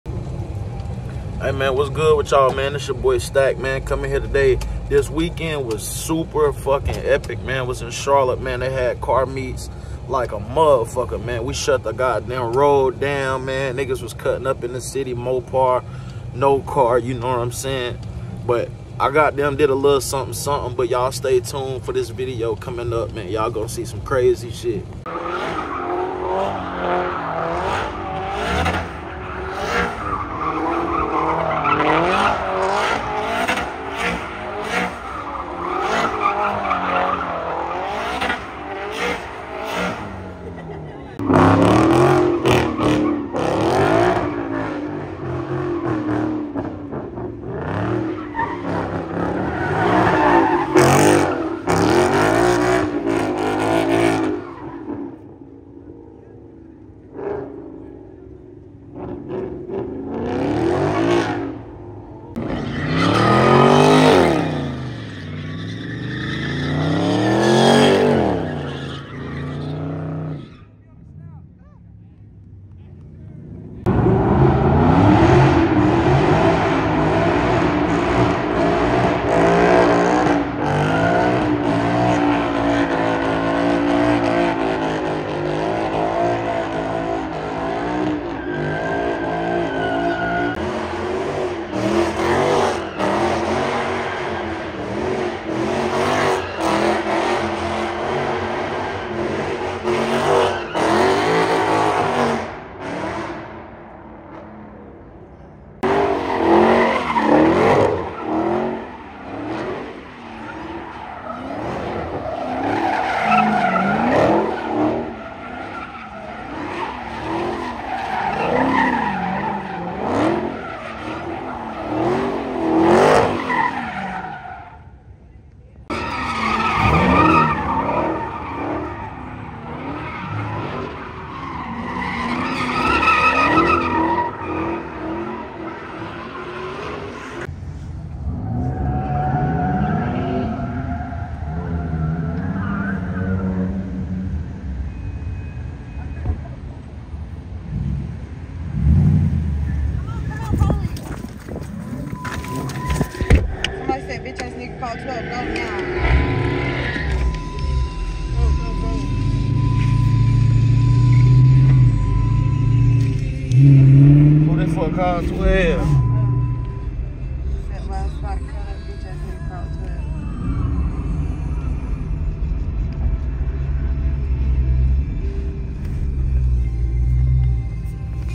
WILD CAR MEET - BURNOUTS & DONUTS (CHARLOTTE NC)
Donuts, Burn outs, drag racing all in Charlotte Nc at this crazy car meet gone wild.